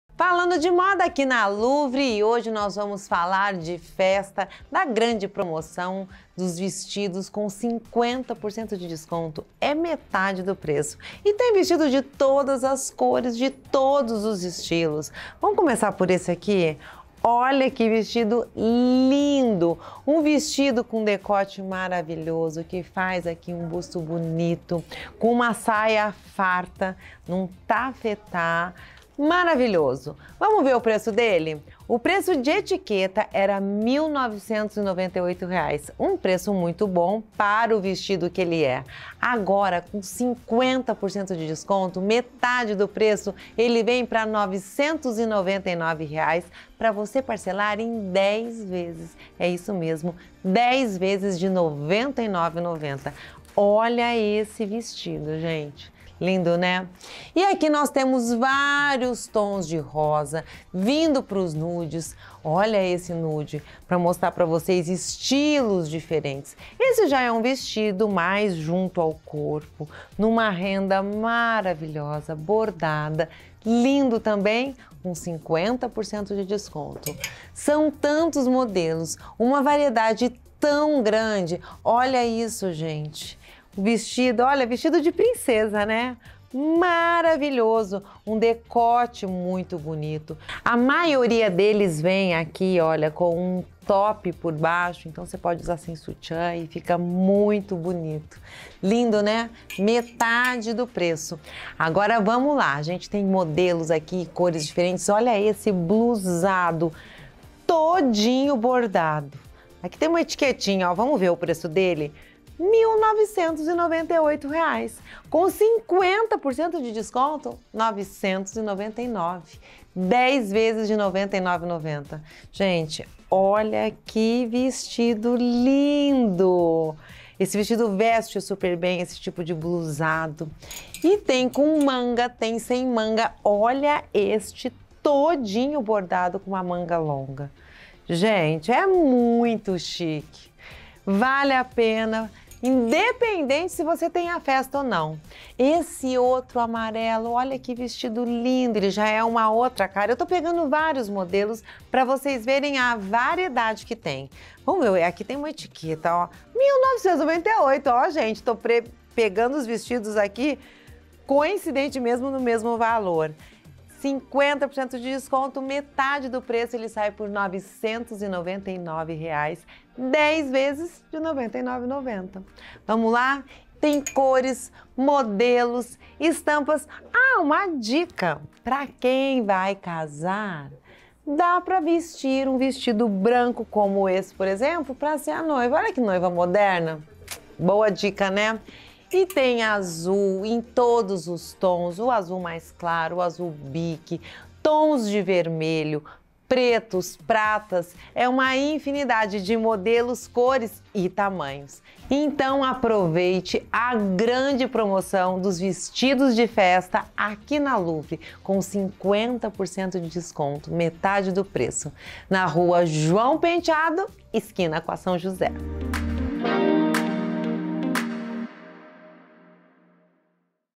VENHA CONFERIR OS VESTIDOS DE FESTAS INCRÍVEIS DA LOUVRE QUE ESTÃO COM 50% OFF 😍| Lelinha Gentil
Confira todos os detalhes dos melhores vestidos de festas da Louvre que estão com 50% de desconto! Aproveite para garantir as melhores peças.😍